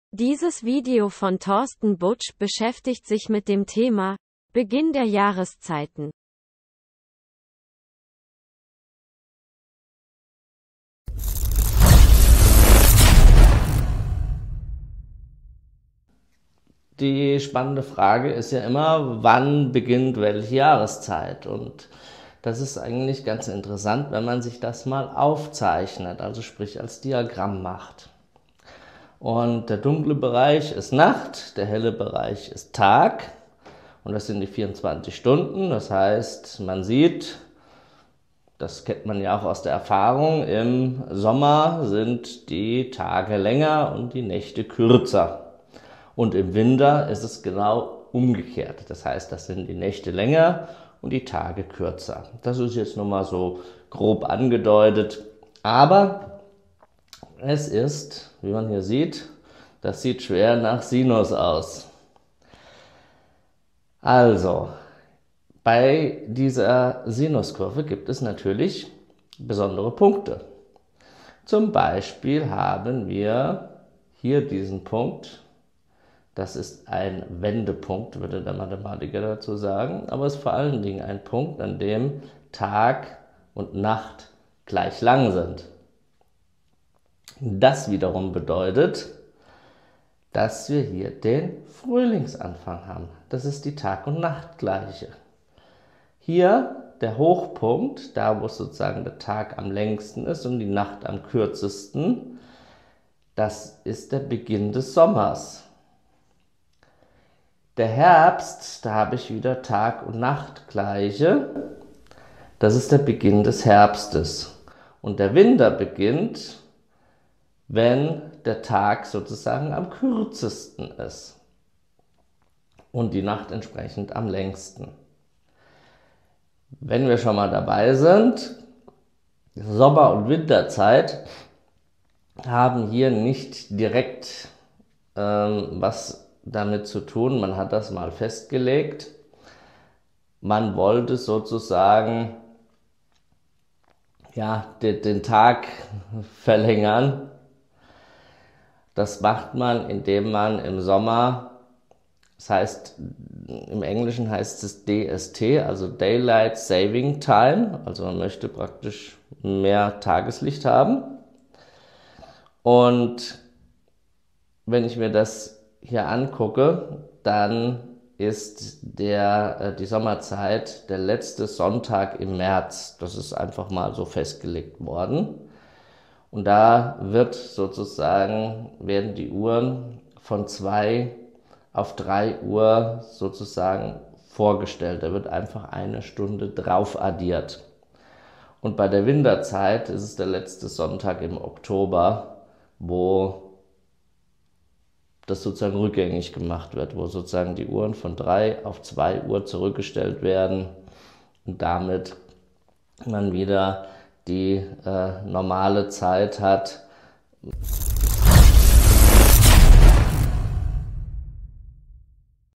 Physik (Grundwissen) Beginn der Jahreszeiten
In diesem Physik-Video erfährst du alles über Beginn der Jahreszeiten. Viel Spaß damit.

Konstruktive Kritik und Kommentare sind immer willkommen. Falls dir das Video gefällt, würde ich mich über ein Like oder ein Abo freuen.